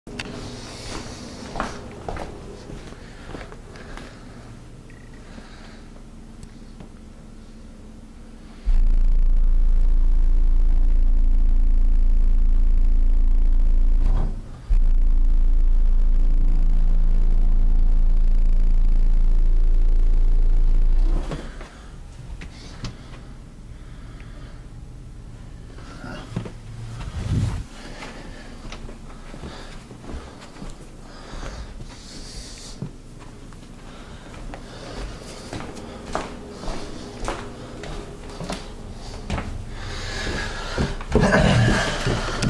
TERRORIZING Gempalm's house PART TWO. Must watch shaking house.
Shaking his house this is a 1080 p version with different tones.  I was parked 10 ft in front of his garage hes up on the second story recording.  As well this car easily shakes his house still from across the street.